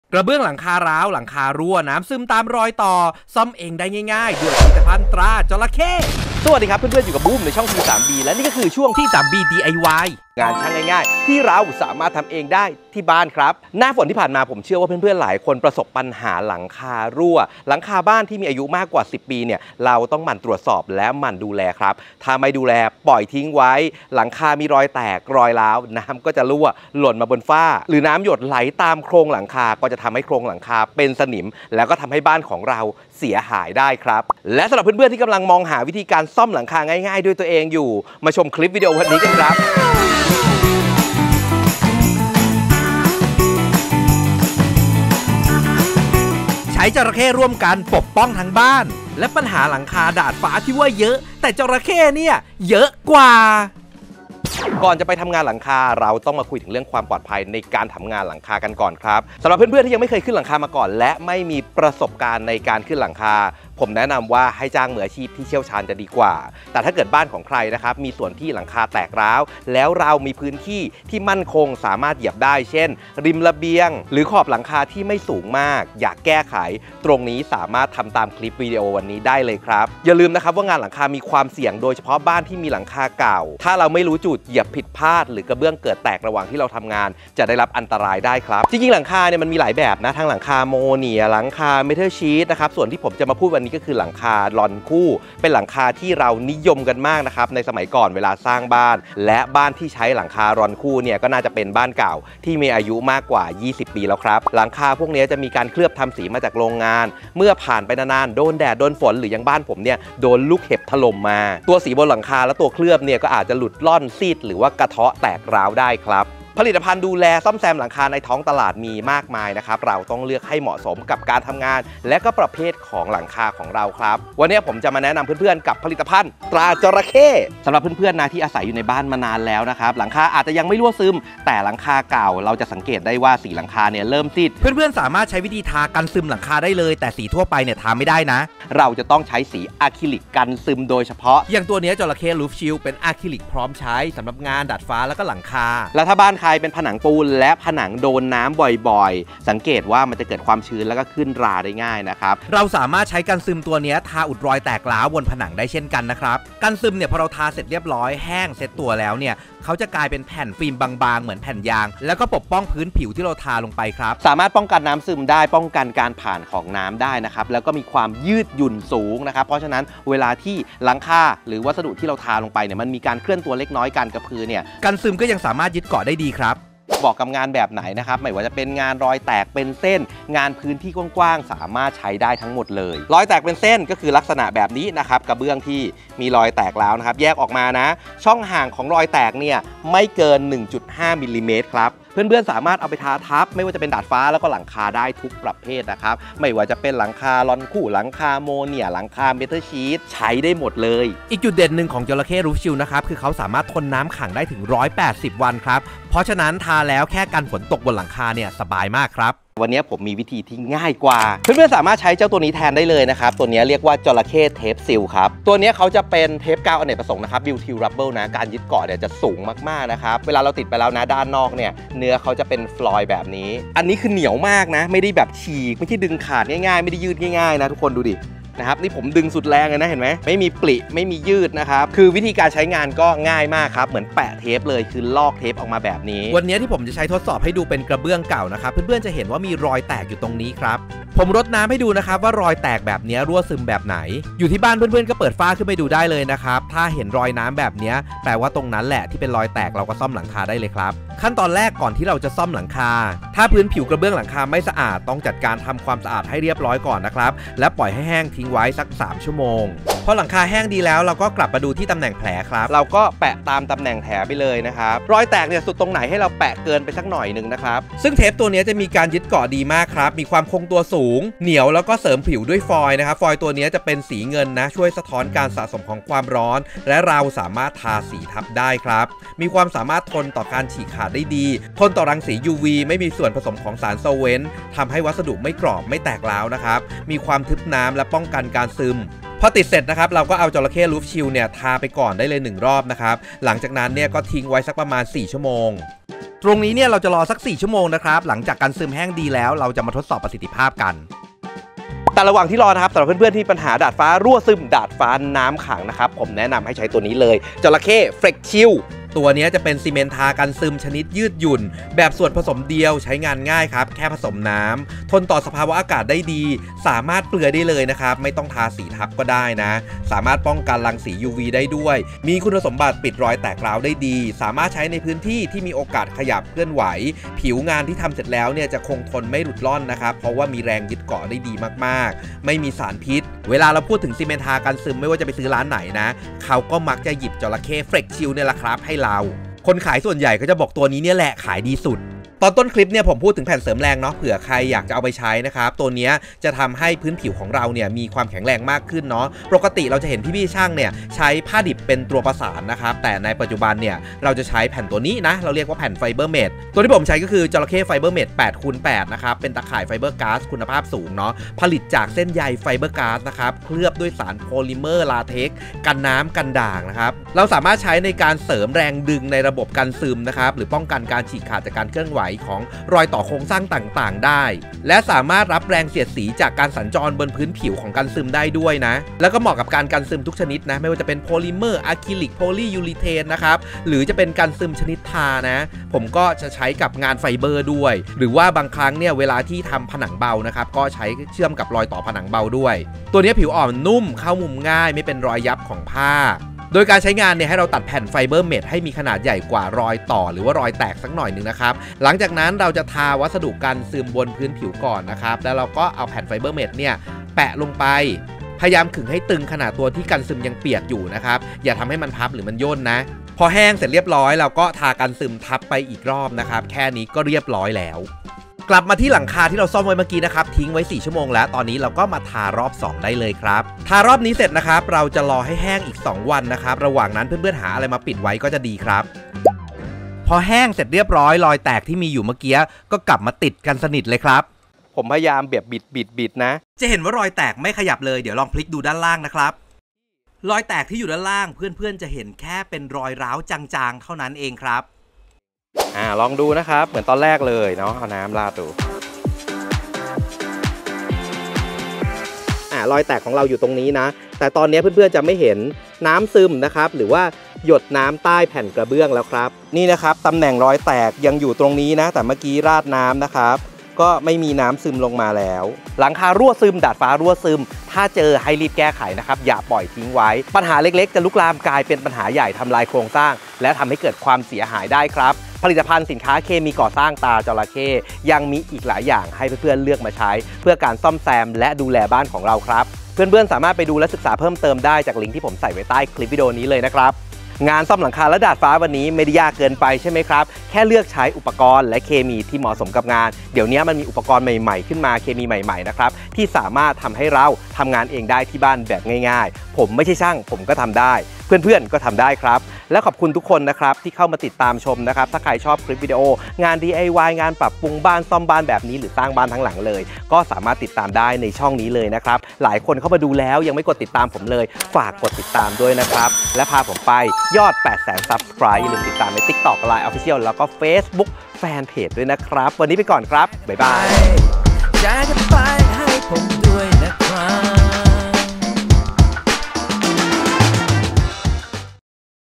วิธี ซ่อมหลังคารั่ว ด้วยตัวเอง ง่ายไม่ต้องรื้อกระเบื้อง T3B DIY
งานซ่อมหลังคา ดาดฟ้ารั่ว ไม่ใช่เรื่องยาก
หากใช้ผลิตภัณฑ์ เคมีที่ถูกต้อง
ผลิตภัณฑ์ตราจระเข้ มีให้เลือกตามความต้องการ
ใช้งานง่าย เหมาะสำหรับ DIY และ พี่ช่างมืออาชีพ
งานหลังคามีความเสี่ยง ต้องทำอย่างระมัดระวัง

รับรองคำถามจะได้รับคำตอบก่อนใคร